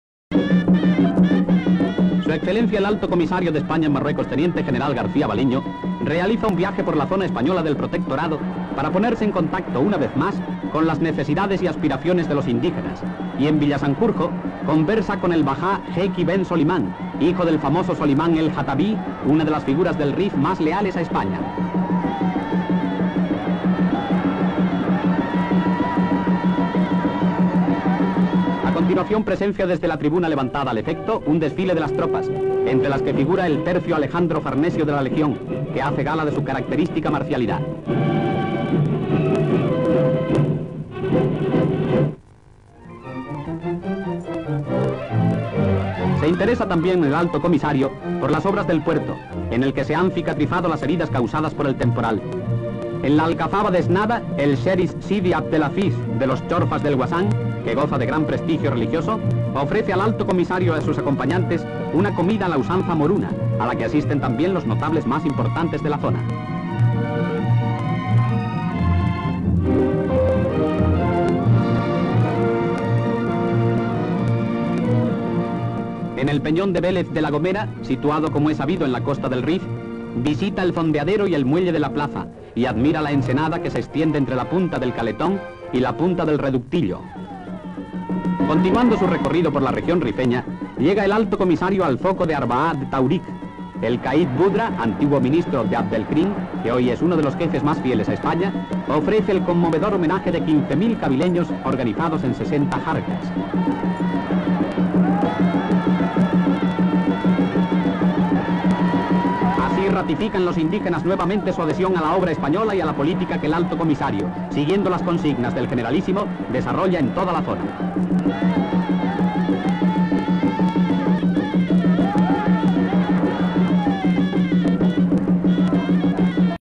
زيارة المستعمر الجنرال كَارسيا فالينيو لمنطقة الريف واستقباله من طرف بعض خونة الريف
فلم وثائقي لزيارت المستعمر الاسباني الى بلاد الريف من تلفزة الإسبانية